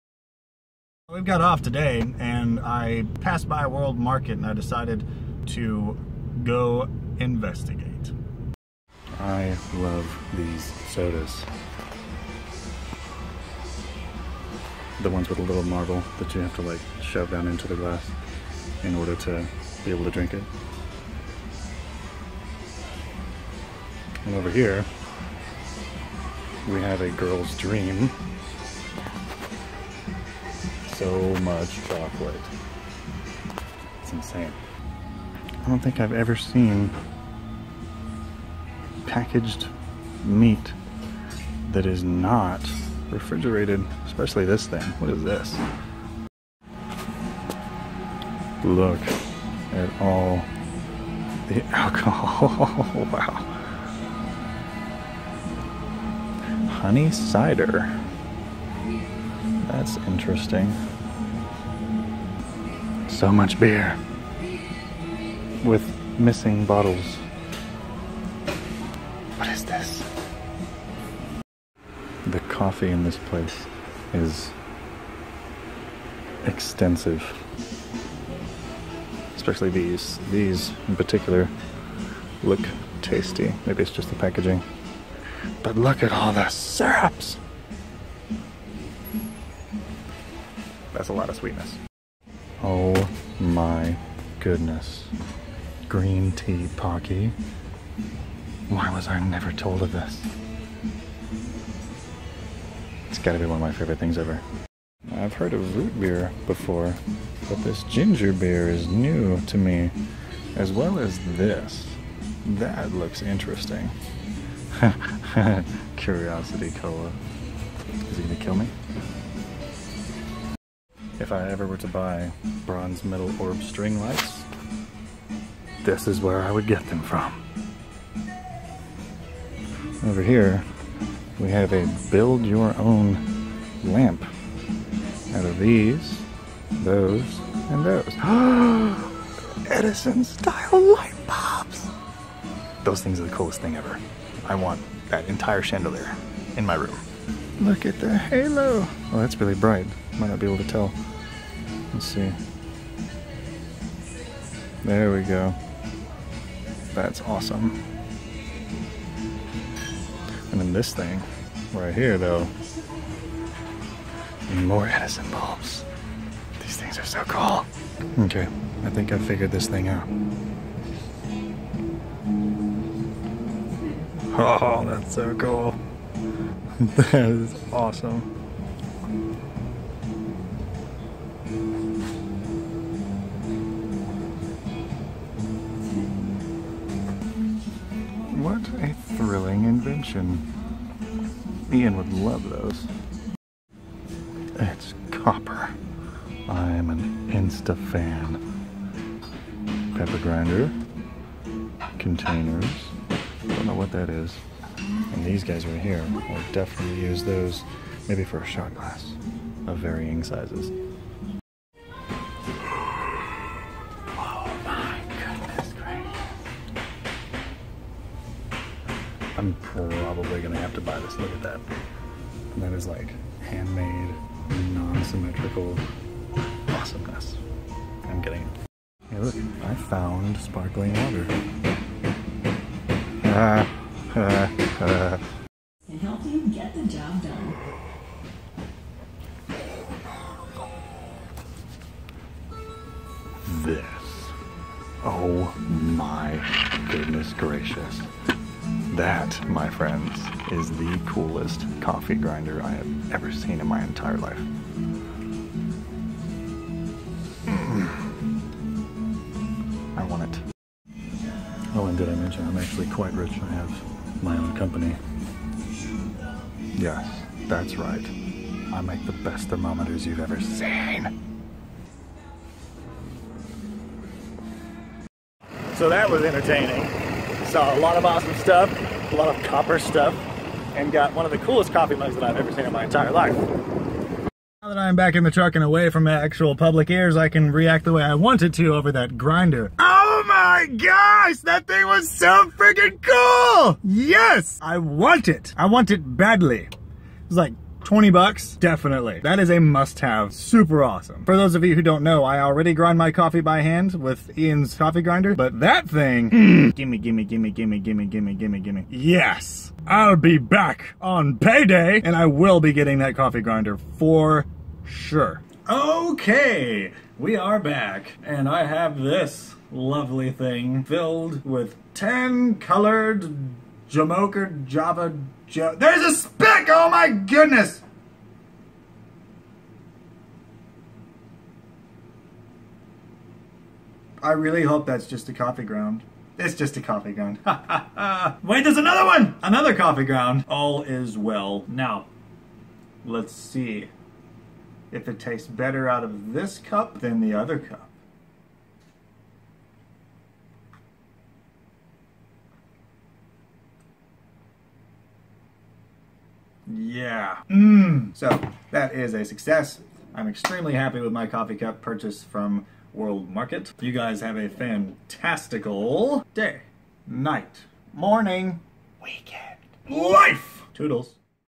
Taylor Goes To World Market (Amazing Coffee Grinder)
Found one of the coolest coffee cups there too, as well as a magic paperweight...

Tried to boost the audio where it needed it but there could still be some quiet parts, depending on how loud you're playing it.

And who the hell keeps BANGING!?